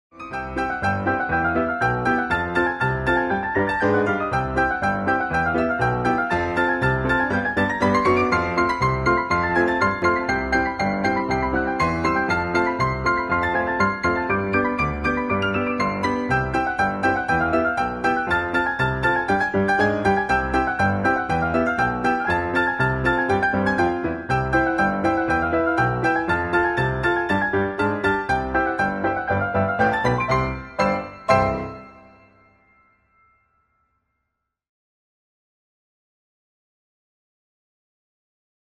The Thief
A silent film written and performed by Drama by George students during our Spring 2015 MovieMakers Drama Club at Stopher Elementary School.